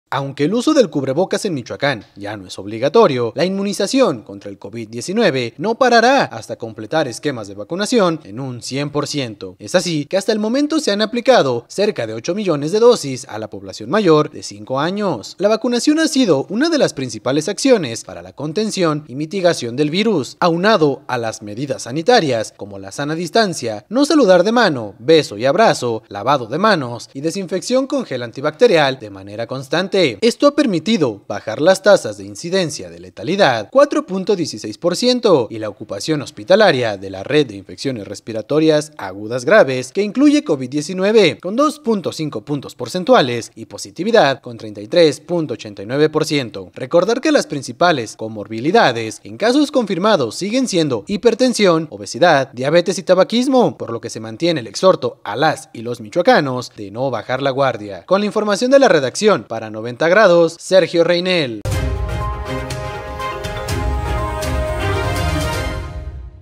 Vacunación contra Covid 19 en Michoacán no parará
Aunque el uso del cubrebocas en Michoacán ya no es obligatorio, la inmunización contra el COVID-19 no parará hasta completar esquemas de vacunación en un 100 por ciento, es así que hasta el momento se han aplicado cerca de 8 millones de dosis a población mayor de 5 años.